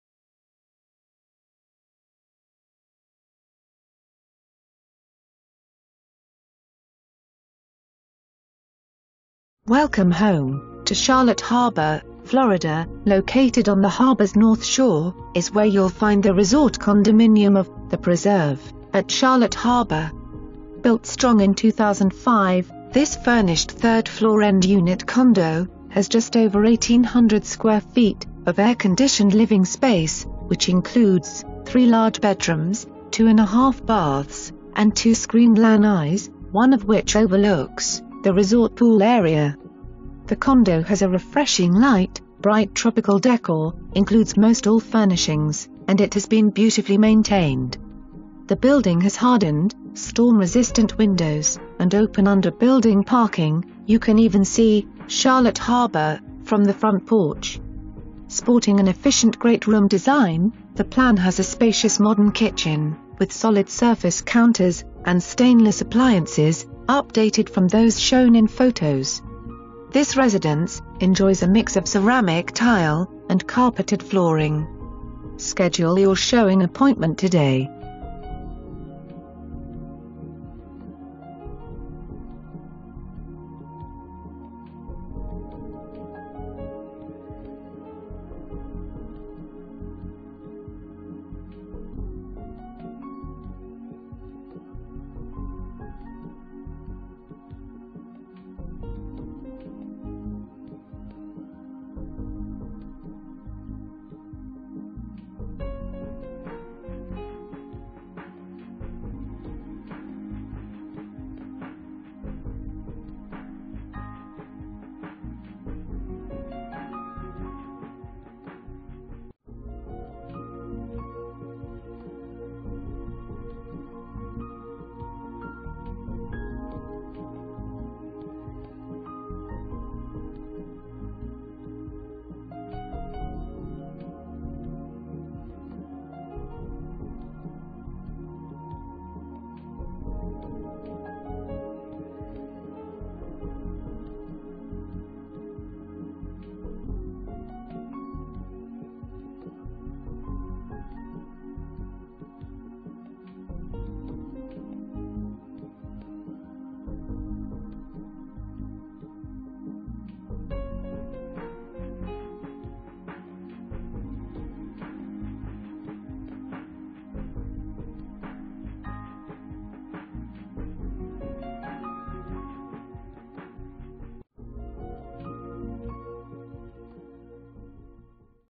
5119 Melbourne Street
Welcome home to Charlotte Harbor, Florida – Located on the harbor’s north shore is where you’ll find the resort condominium of The Preserve at Charlotte Harbor. Built strong in 2005, this FURNISHED 3rd floor end unit has just over 1800 square feet of air conditioned living space which includes 3 large bedrooms, 2.5 baths and 2 screened lanais, one of which overlooks the resort pool area. This condo has a refreshing light, bright tropical décor (include most all furnishings) and it has been beautifully maintained. The building has hardened, storm resistant windows and open under-building parking; you can even see Charlotte harbor from the front porch! Sporting an efficient great room design the plan has a spacious modern kitchen with solid surface counters and stainless appliances (updated from those shown in photos). The residence enjoys a mix of ceramic tile and carpet flooring. Schedule your showing today!

Presented By:
The MLS Detectives, RE/MAX Anchor Realty